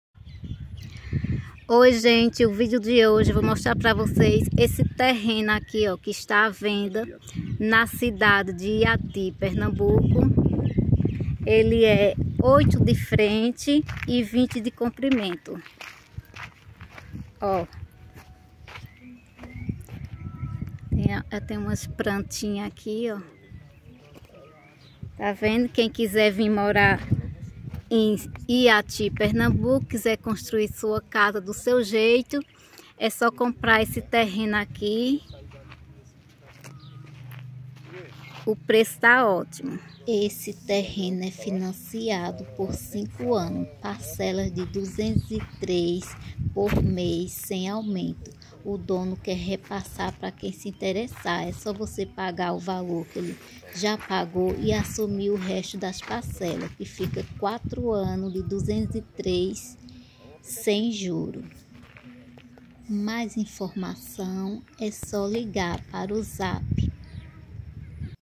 vende-se esse terreno financiado sua chance de ter o seu é agora
oi pessoal esse terreno é financiado o dono já pagou 3,400 ,se você se interessa é só ligar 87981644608👍